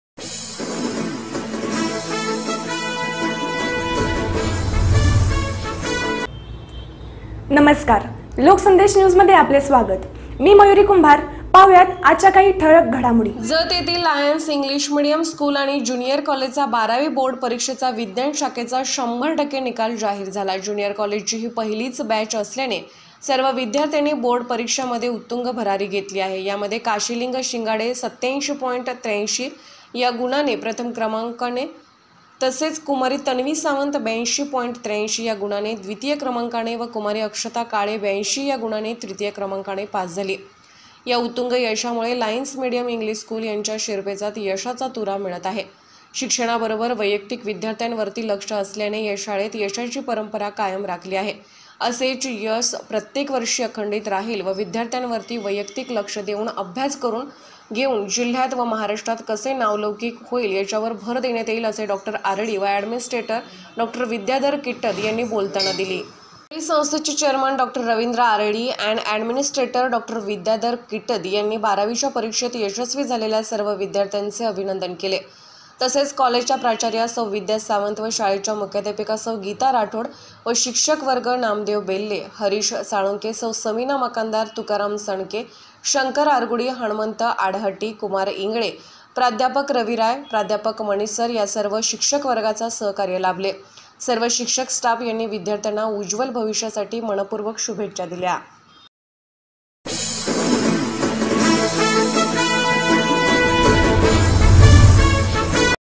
लायन्स इंग्लिश मेडियम स्कूल आणि ज्युनिअर कॉलेज, जतचा १२ वी बोर्ड परीक्षेचा १००% निकाल
LOKSANDESH : लायन्स इंग्लिश मेडियम स्कूल आणि ज्युनिअर कॉलेज, जतचा १२ वी बोर्ड परीक्षेचा १००% निकाल.

(काळ्या पट्टीत )जत तालुक्यात लाईन्स इंग्लिश मीडियम स्कूल चे विद्यार्थी अव्वल